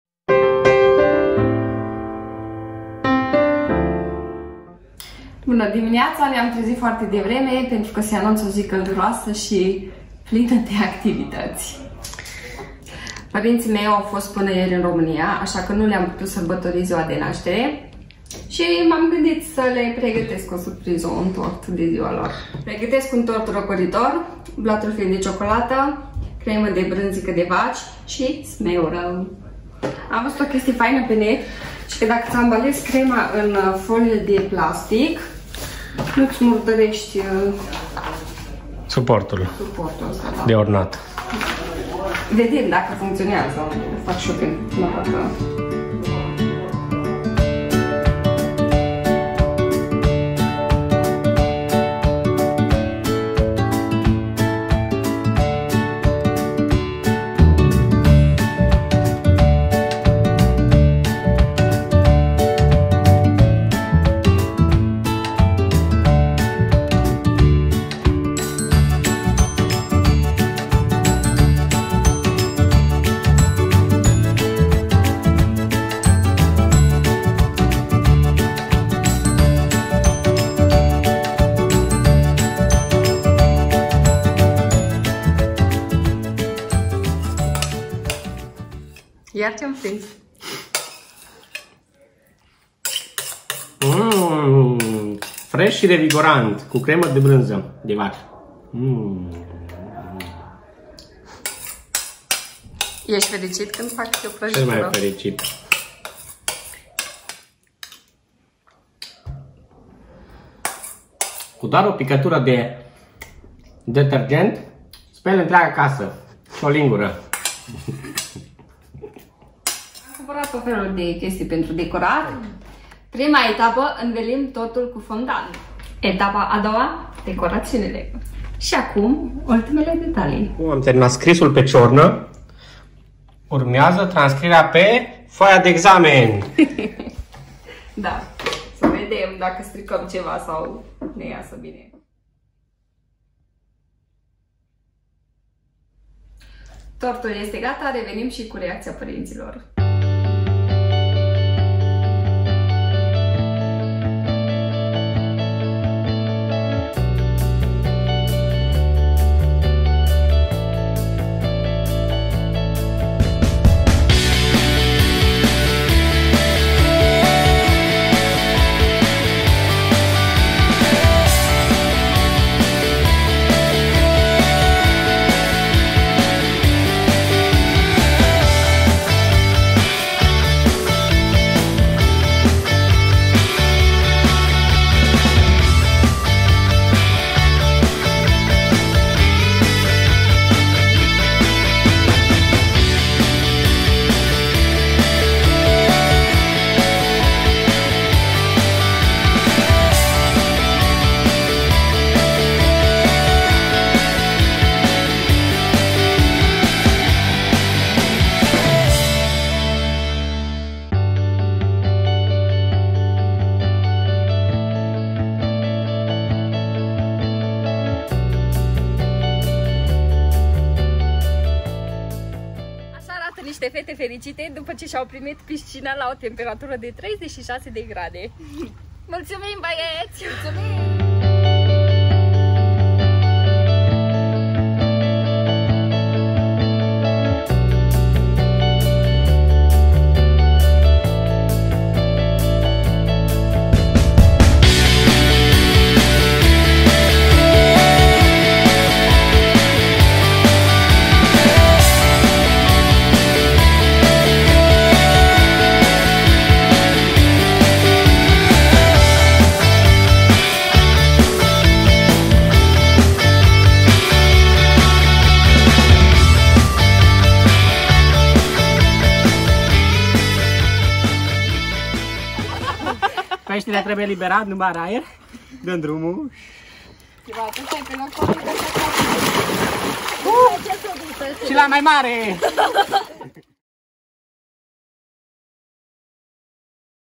Le-a placut surpriza noastra!
Ne-am gandit sa le facem o surpriza parintilor nostri, asa ca am pregatit cateva cadouri si un tort pentru ziua lor de nastere. Am incheiat ziua cu o baie in "piscina".
Speram sa va placa video-ul! :)